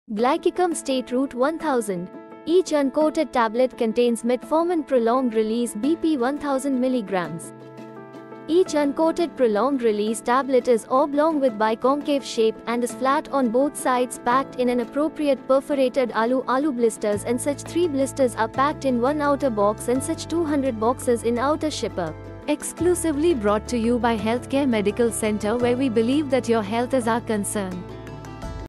Glycikam SR 1000
Each uncoated prolonged release tablet is oblong with biconcave shape and is flat on both sides packed in an appropriate perforated Alu Alu Blisters and such 3 blisters are packed in one outer box and such 200 boxes in outer shipper.